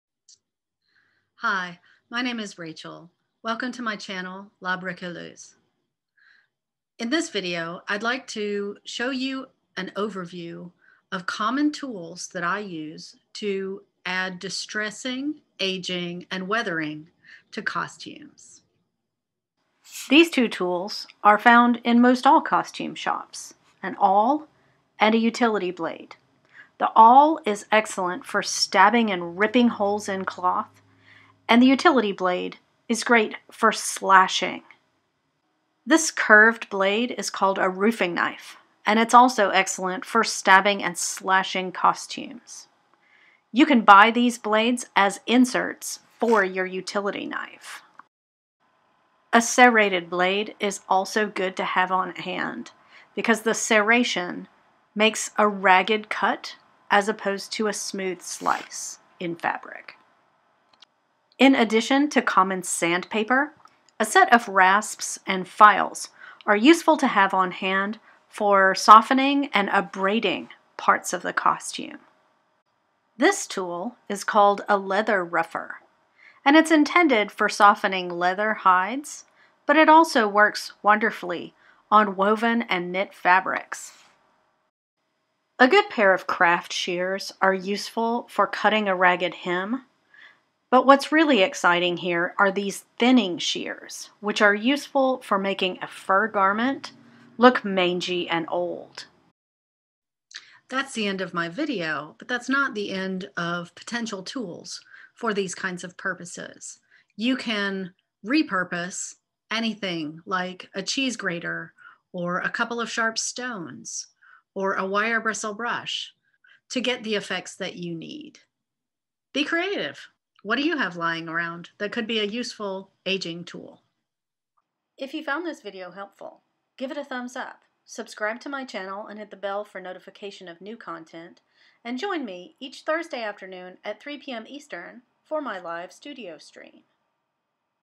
Tools for aging, distressing, and weathering costumes
In this video, I show you several common tools you may already have that you can use for distressing costumes, weathering costumes & making costumes look old, and also a few specialized ones you might want to acquire!

After you sew the perfect costume, sometimes distressing is what makes it look real!